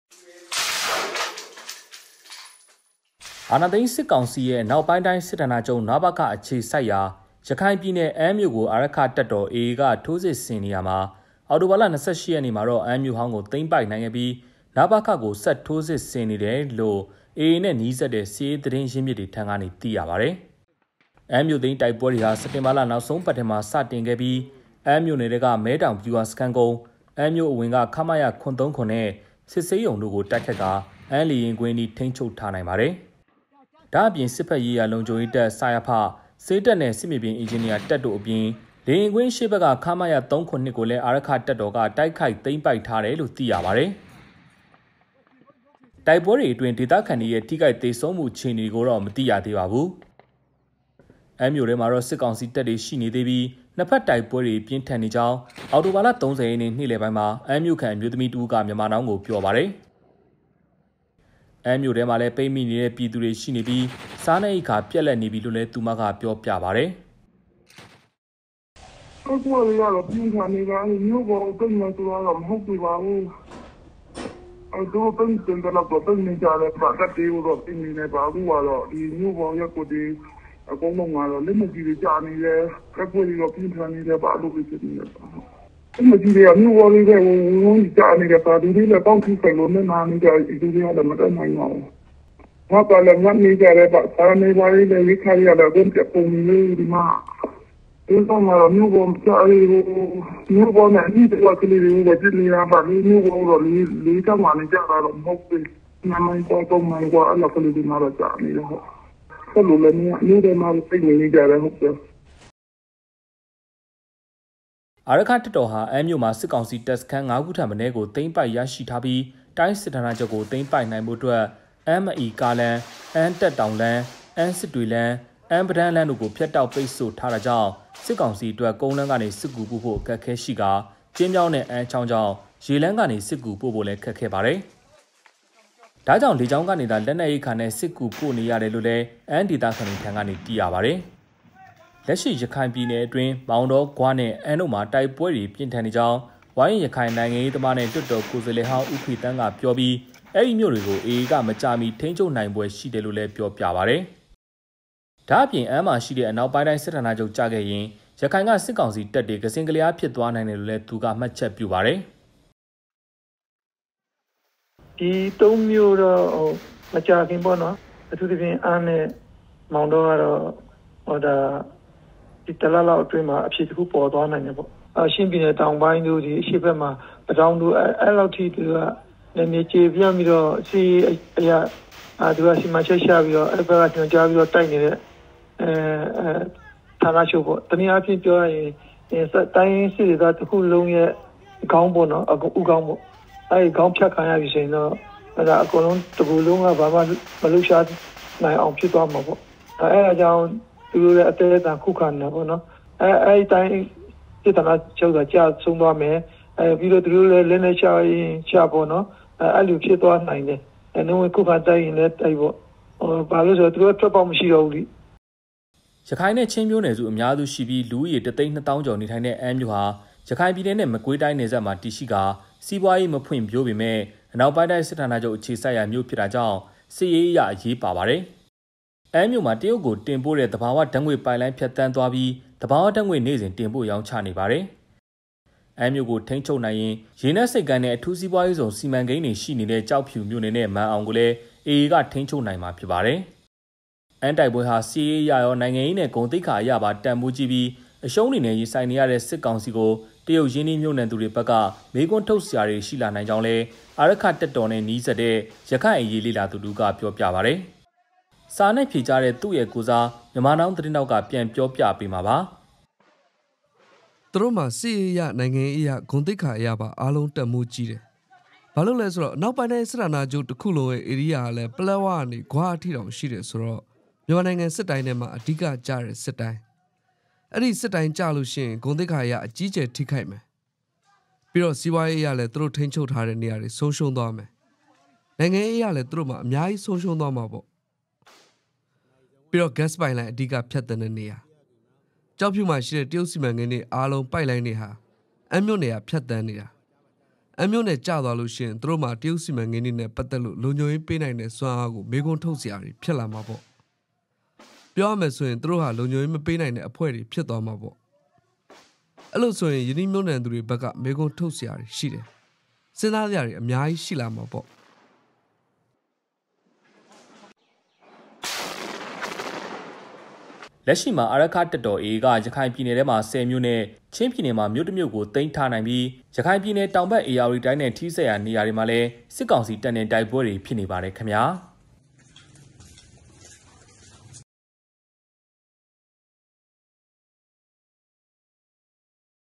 အနောက်ပိုင်းတိုင်းစစ်ဌာနချုပ်ရှိရာ အမ်းမြို့သိမ်းတိုက်ပွဲ ဘယ်လောက်ထိ အရေးပါလဲ
အနောက်ပိုင်းတိုင်းစစ်ဌာနချုပ်(နပခ) တည်ရှိရာ ရခိုင်ပြည်နယ်၊ အမ်းမြို့ မြို့သိမ်းတိုက်ပွဲကို အာရက္ခတပ်တော် AA က ဝိုင်းပတ်ပိတ်ဆို့ကာ အရှိန်မြှင့်တိုက်ခိုက်နေရာ နပခ ဌာနချုပ်အပါအဝင် အမ်းမြို့နယ်တစ်ခုလုံးကို စစ်ကောင်စီက မကြာခင် လက်လွှတ်ရဖွယ်ရှိနေသည်ဟု ဒေသခံများနှင့် စစ်ရေးအကဲခတ်များက သုံးသပ်နေကြသည်။

အမ်းမြို့ကို လက်မလွှတ်ရရေးသည် စစ်ကောင်စီအတွက် နိုင်ငံရေး၊ စစ်ရေးသာမက စီးပွားရေးအတွက်ပါ အရေးပါနေသည်။

“ကျောက်ဖြူမှာရှိတဲ့ တရုတ်စီမံကိန်းတွေအားလုံး ပိုက်လိုင်းတွေဟာ အမ်းမြို့နယ်က ဖြတ်သန်းနေ တာ။ အမ်းမြို့နယ်ကျသွားရင် သူတို့မှာ တရုတ်စီမံကိန်းတွေနဲ့ပတ်သက်လို့ လုံခြုံရေးပေးနိုင်တဲ့ စွမ်းအားကို မေးခွန်းထုတ်စရာတွေဖြစ်လာမှာပေါ့။ ပြောရမယ်ဆိုရင် သူတို့ဟာ လုံခြုံရေးမပေးနိုင်တဲ့ အဖွဲ့တွေဖြစ်သွားမှာပေါ့ ။အဲလိုဆိုရင်တော့ ရင်းနှီးမြုပ်နှံသူတွေဘက်က မေးခွန်း ထုတ်စရာတွေ ရှိတယ်။ စဥ်းစားစရာတွေအများကြီးရှိလာမှာပေါ့”  ဟု AA နှင့်နီးစပ်သည့် ရခိုင်နိုင်ငံရေး၊ စစ်ရေးလေ့လာသူက သုံးသပ်သည်။

သတင်း၊ တင်ဆက် -မြတ်ကြီး